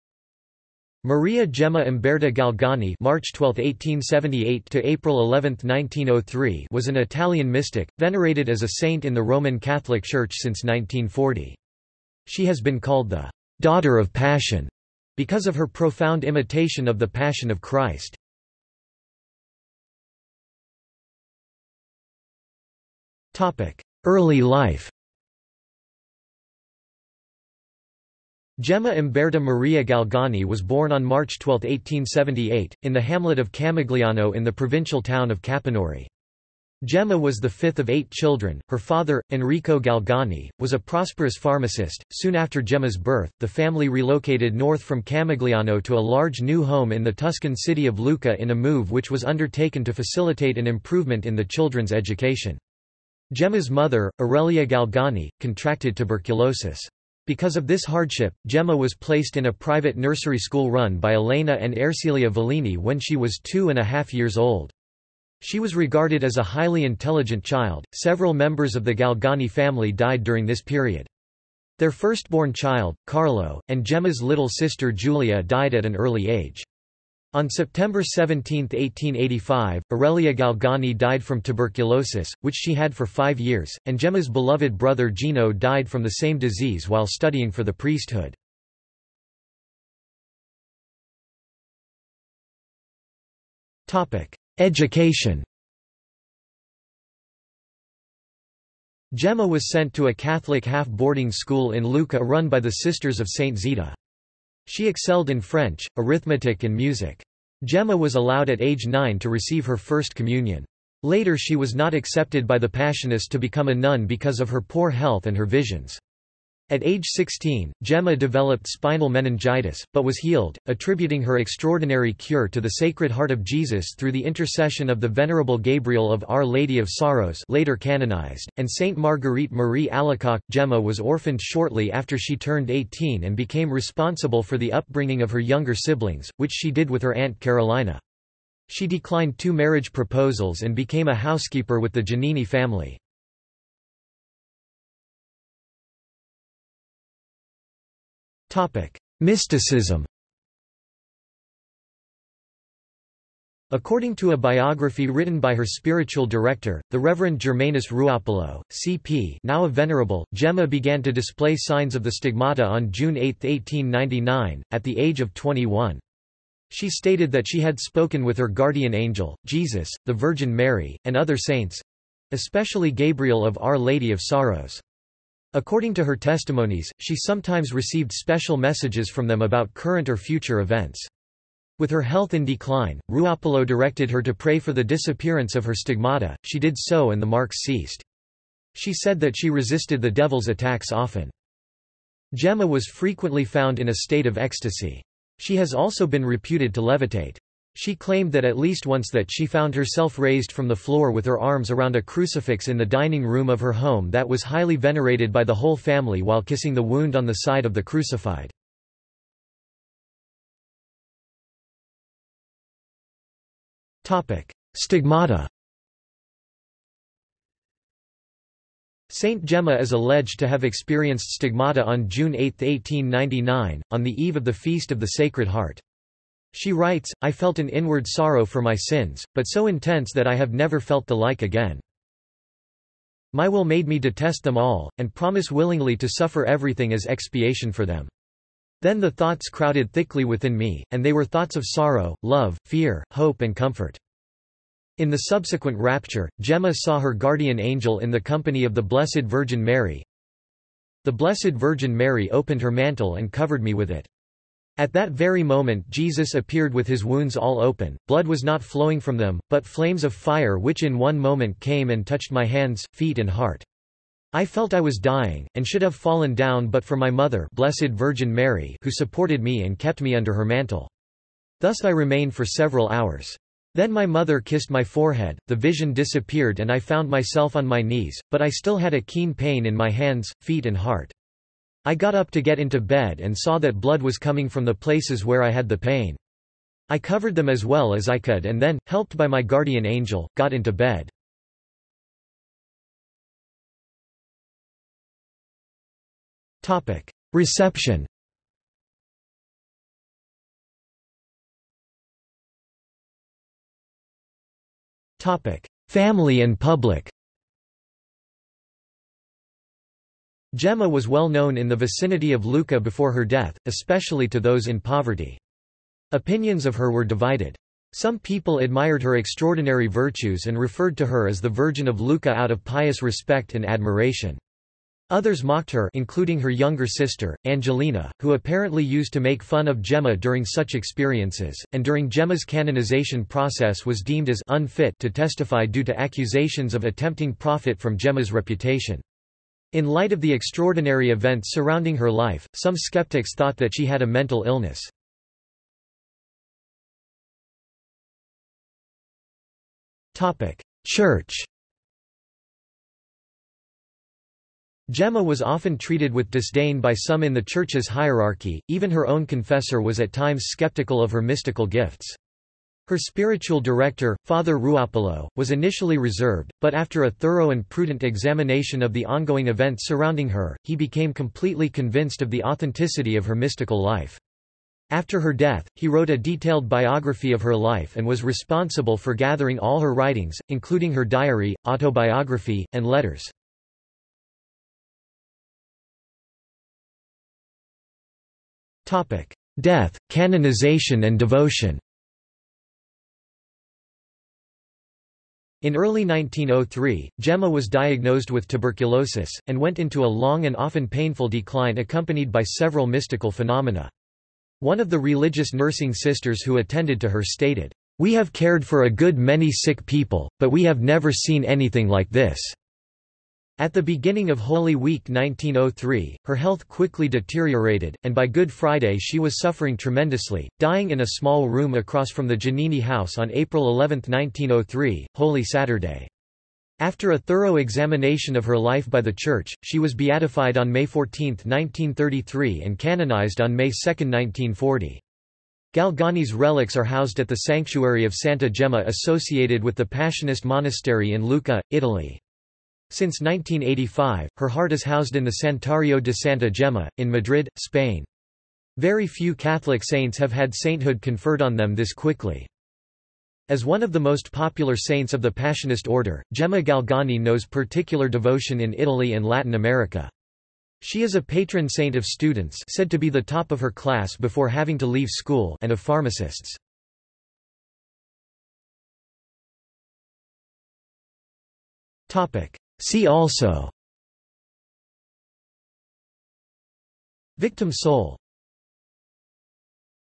Gemma Galgani | Wikipedia audio article
This is an audio version of the Wikipedia Article:
Gemma Galgani

00:00:19 1 Early life
00:01:30 2 Education
00:02:24 3 Mysticism
00:03:30 4 Stigmata
00:04:53 5 Reception
00:05:01 5.1 Family and public
00:05:46 5.2 Church
00:06:25 6 Death, canonization and devotion
00:07:48 7 See also

- Socrates

SUMMARY
Maria Gemma Umberta Galgani (March 12, 1878 – April 11, 1903) was an Italian mystic, venerated as a saint in the Roman Catholic Church since 1940. She has been called the "Daughter of Passion" because of her profound imitation of the Passion of Christ.